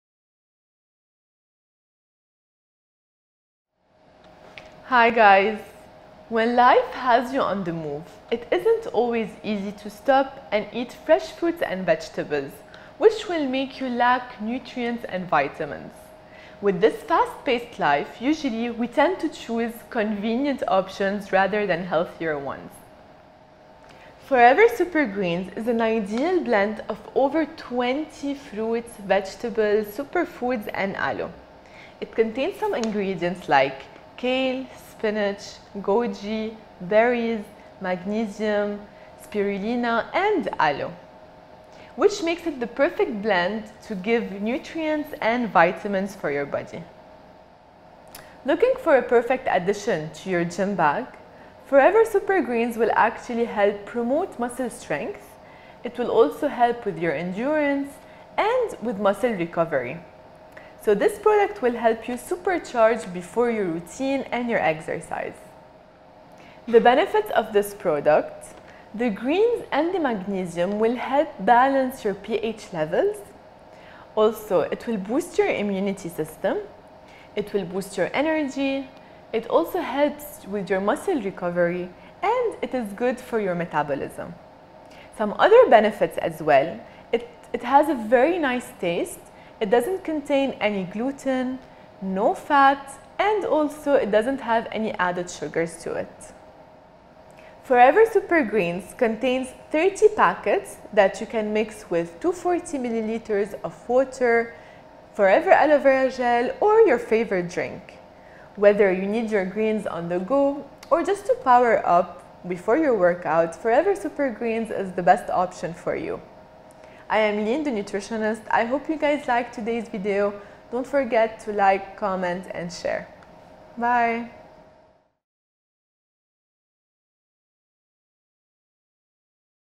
Forever Supergreens | Weekly Advice
Forever Supergreens contains over twenty fruits and vegetables with vitamins C, E and magnesium to help keep your body fully powered for optimal performance*.

*Vitamin C contributes to the normal function of the immune system. Vitamin E contributes to the protection of cells from oxidative stress. Magnesium contributes to normal energy-yielding metabolism, reduction of tiredness and fatigue, electrolyte balance and psychological function.